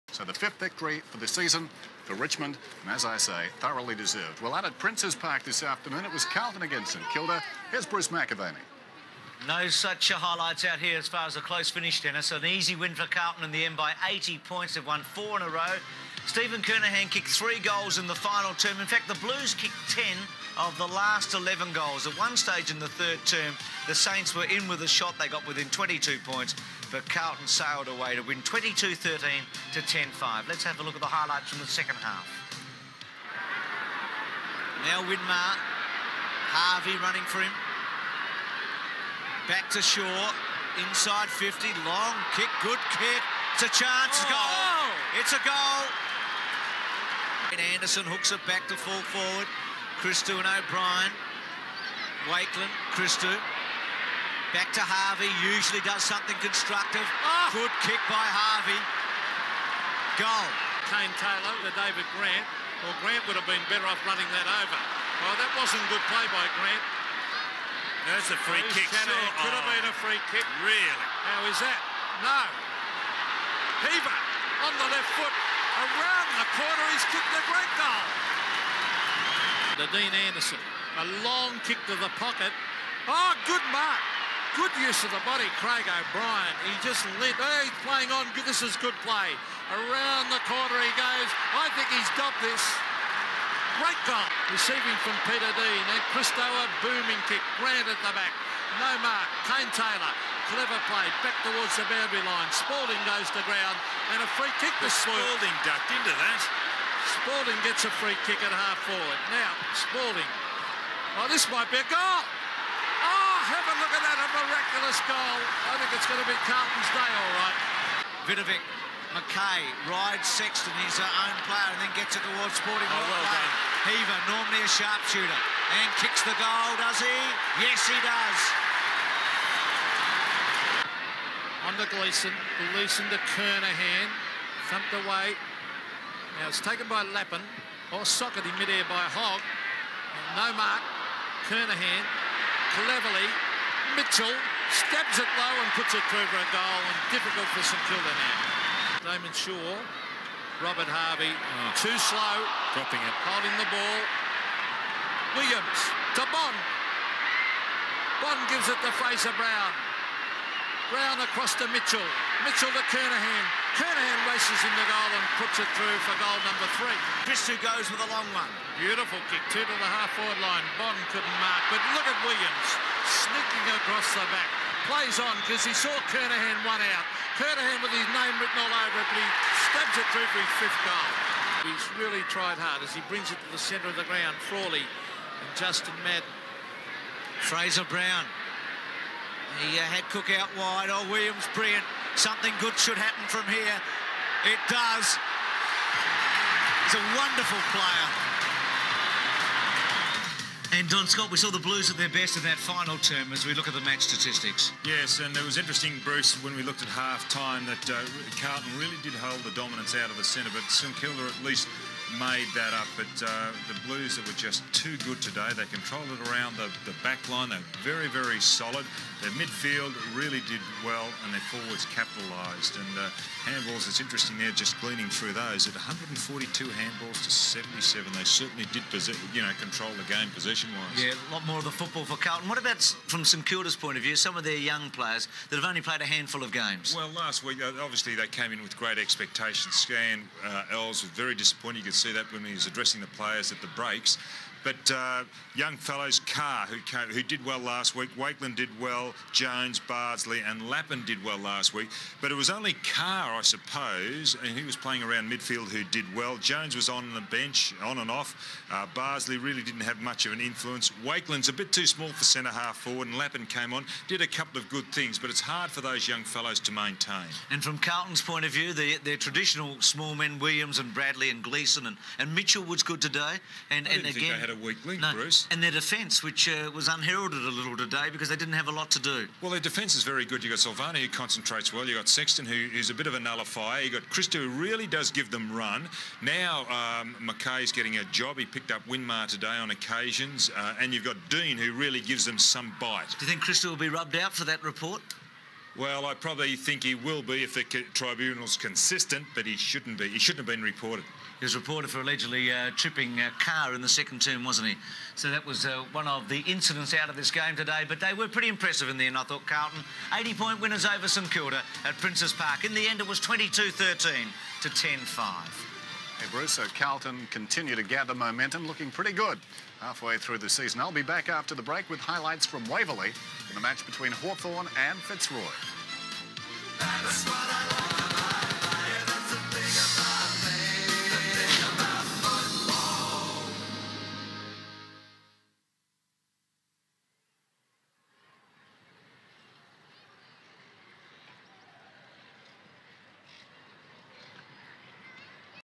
2nd Half Carlton VS St Kilda Robert Harvey Barry Mitchell
2nd half AFL Highlights of the Carlton VS St Kilda match. St Kilda Saints player Robert Harvey and Carlton Blues Barry Mitchell are both featured kicking goals in this video. Below is a link to the 1st half highlight of this match.